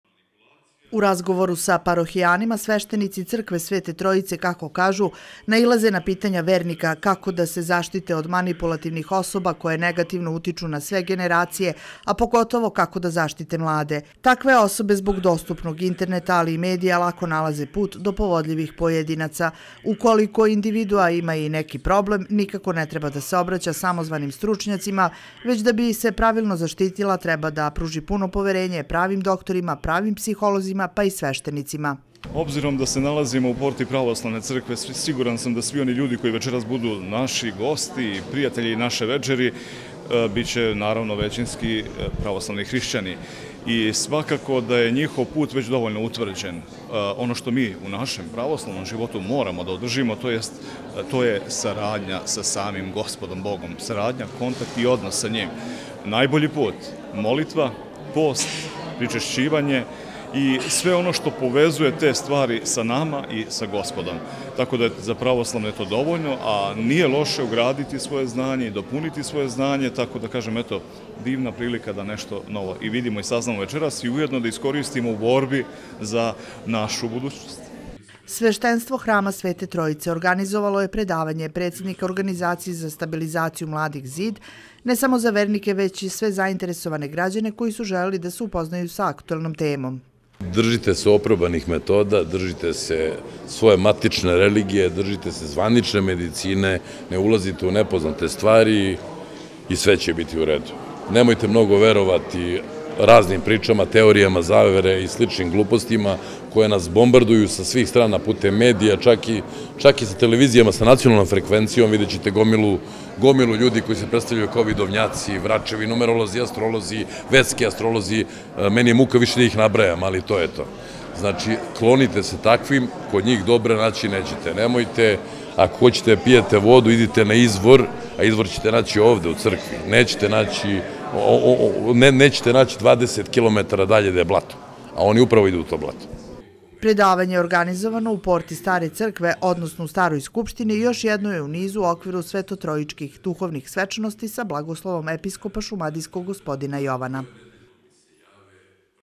Kako se zaštiti od manipulativnih osoba?
Sveštenstvo Hrama SveteTrojice organizovalo je predavanje Dimitrija Pastujovića, predsednika organizacije za stabilizaciju mladih „ Zid“ na temu „Kako prepoznati i zaštiti se od manipulativnih osoba i lažnih duhovnih organizacija“. Predavači su se obraćali ne samo roditeljima, već pojedincima koji i nesvesno u eri medija, čak i sa kanala sa nacionalnih frekfencija mogu biti izloženi potencijalnim strnaputicama.